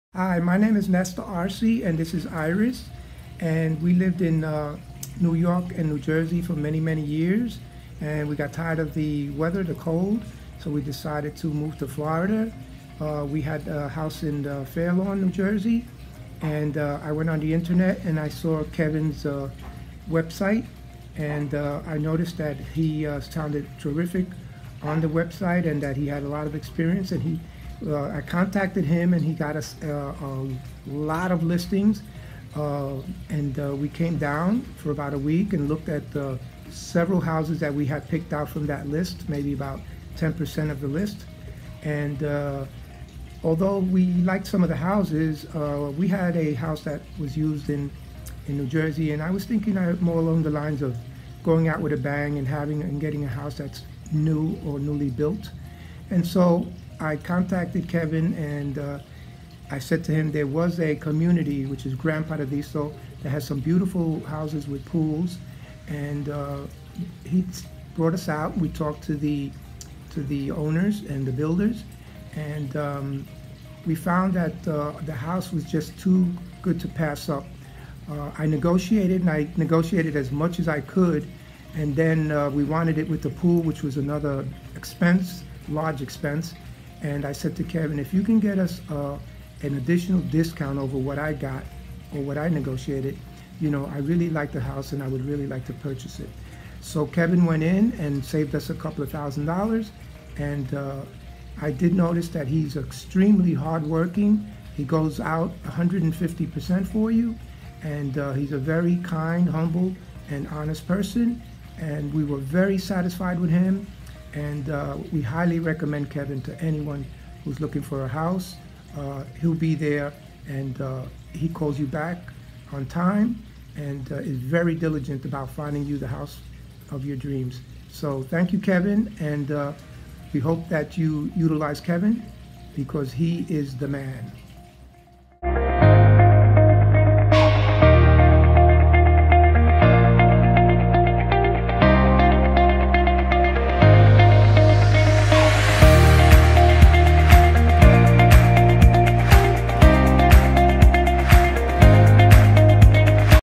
Kevin Tai ‘s Client Testimonial: Nestor and Irish
Thanks Nestor & Irish for the great video and continuous supports! I am really appreciated all the opportunities., I am blessed 😊

Thank you! Kevin Tai (Bright Realty / ⭐️⭐️⭐️⭐️⭐️Premier Agent)